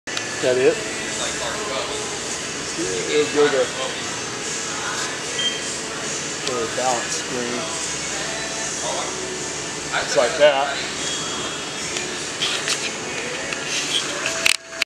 Hunter GSP9700 Balancer, 230v Single Phase Selling on Big Iron Online Auction September 26, 2018